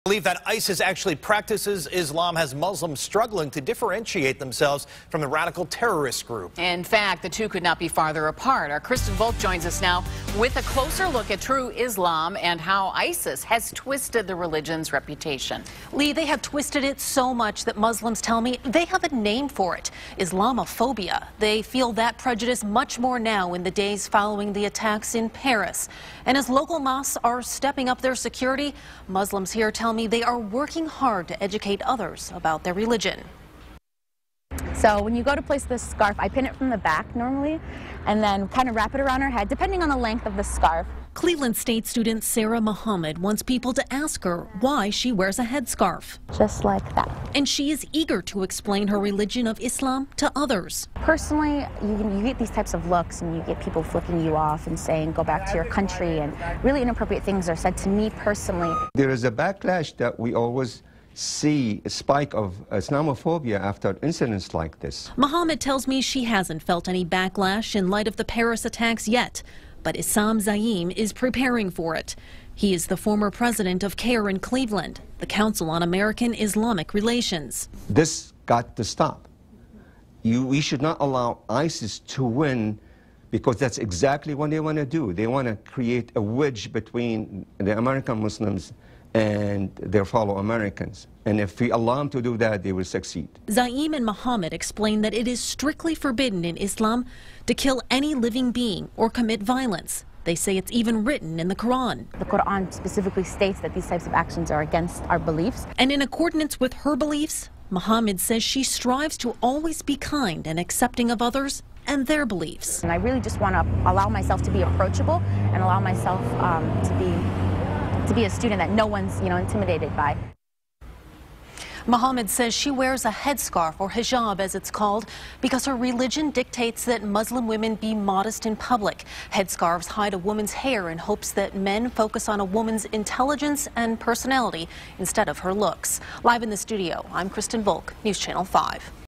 Local Muslims: ISIS is not Islam
Local Muslims fear the backlash after the attacks in Paris. In response, they are trying to educate others about Islam, a religion that strictly forbids violence and promotes peace and love.